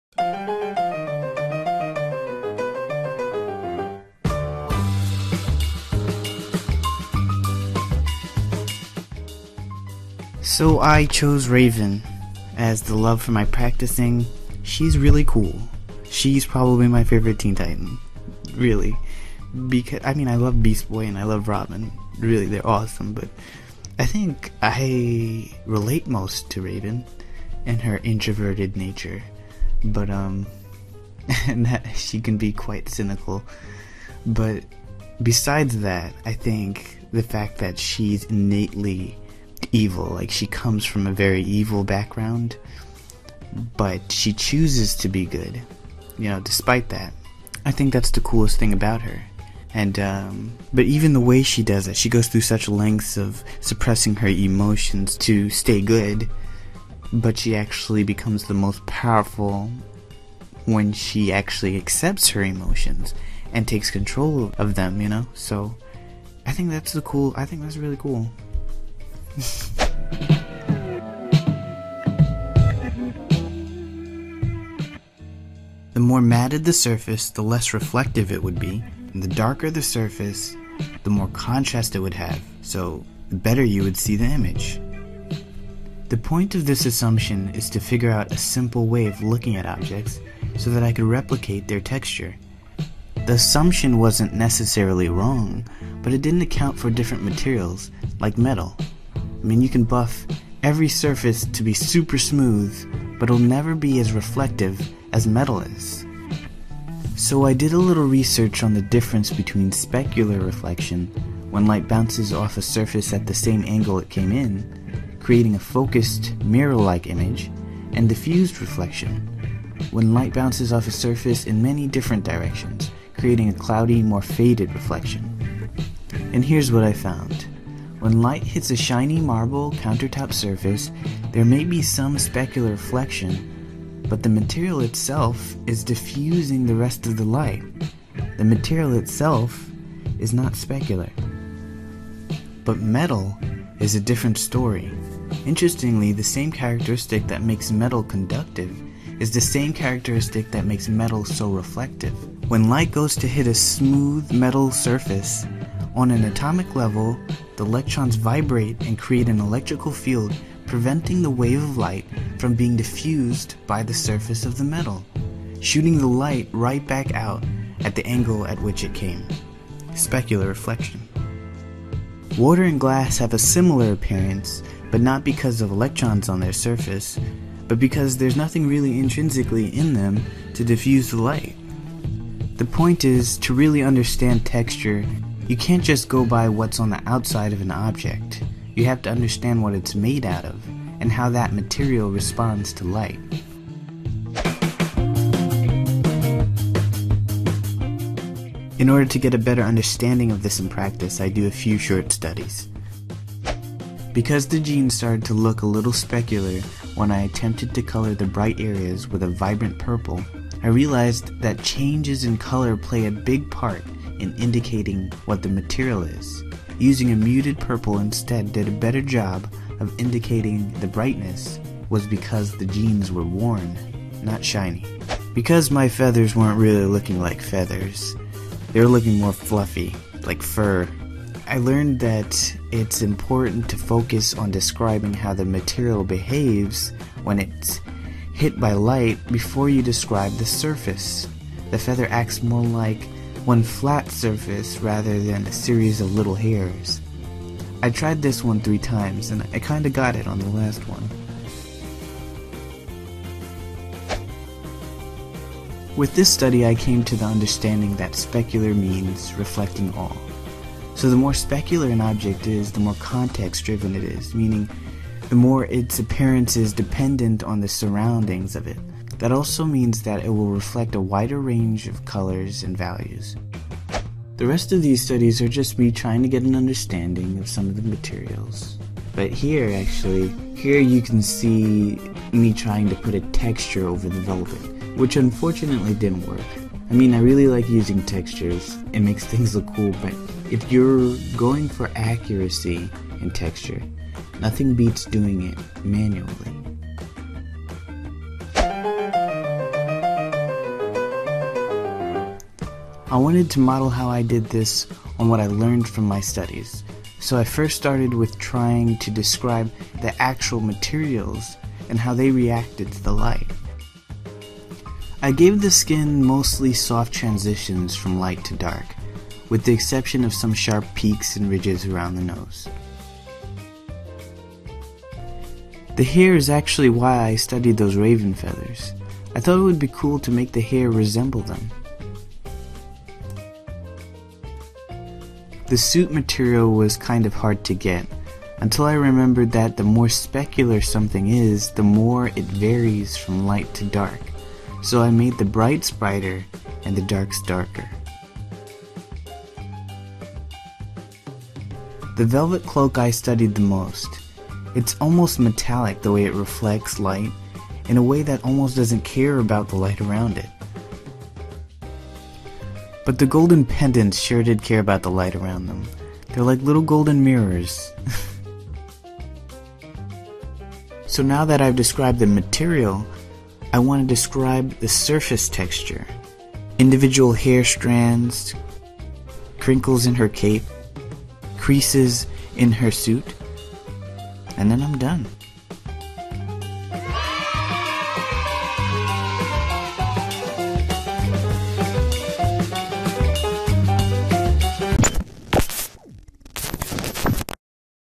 Raven Texture Study : Practice and Love
The first of many in a series. THE PRACTICE: Texture. THE LOVE. Raven Roth. From the Teen Titans, the animated series.

I do a little explaining about how light interacts with different surfaces. To understand texture you have to understand more than just the surface. You have to understand the material as well. Sort of an 'inner texture'. Hope you like it

Here's a link a reference graphic I did on the same idea.

Here's the finished product